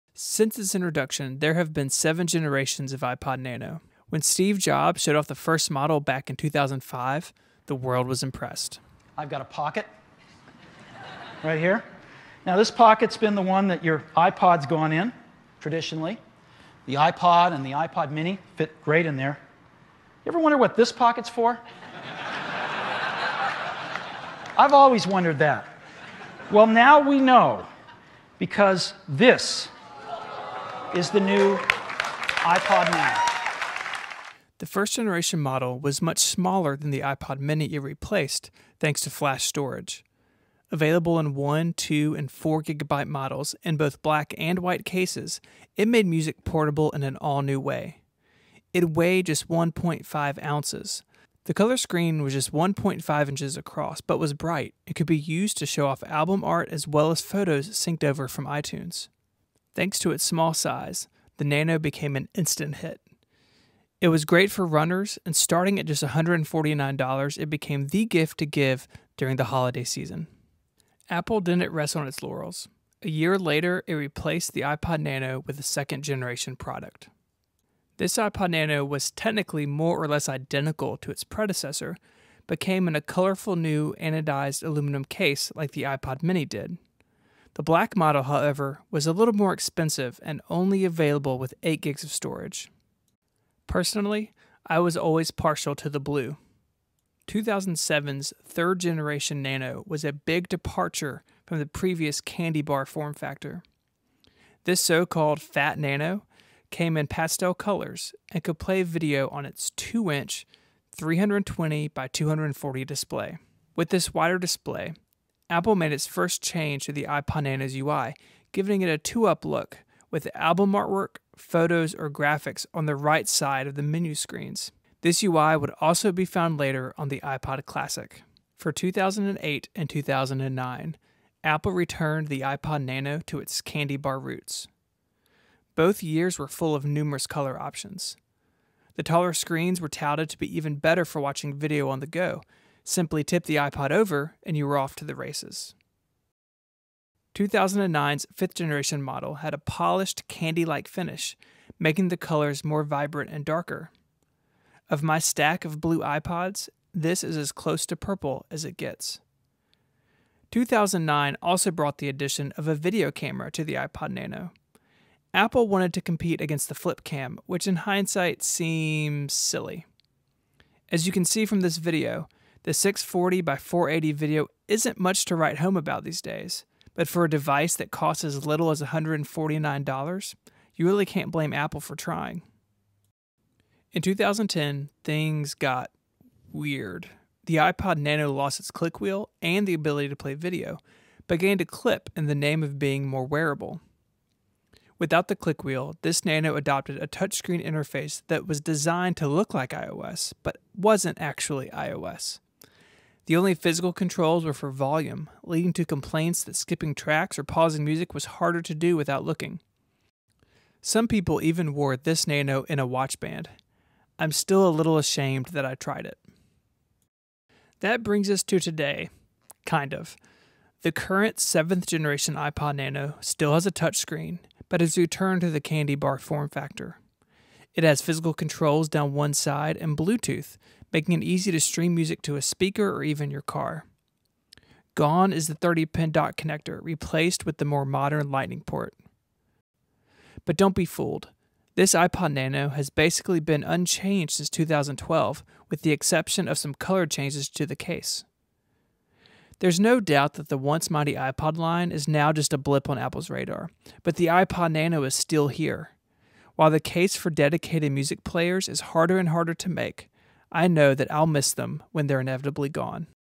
The history of the iPod nano
Since its introduction in 2005, there have been several generations of iPod nano. Apple's small music player has been at the heart of the company's music strategy for over a decade, and has seen a lot of changes in its lifetime.